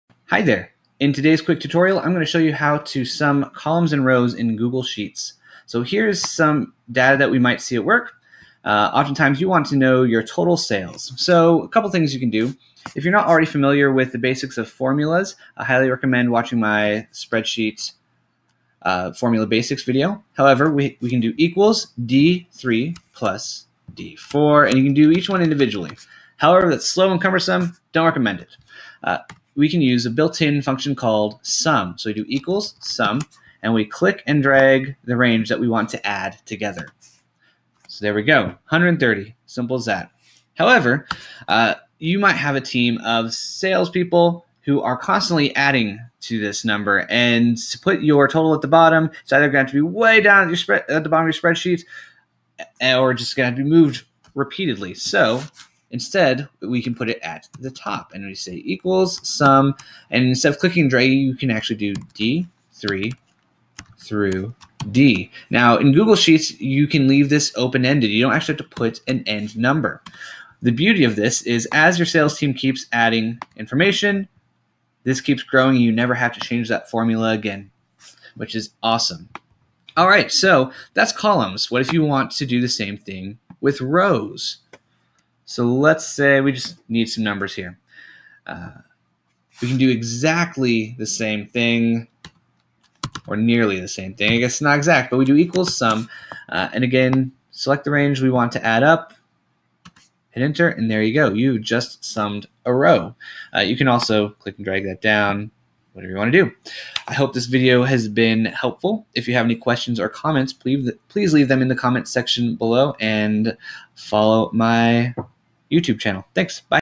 How to sum columns and rows | Google Sheets | Quick Tutorial
In this video, I demonstrate how to sum a column or row of sales data. If you are not familiar with the basics of spreadsheet formulas, check out my Spreadsheet Formula Basics video at: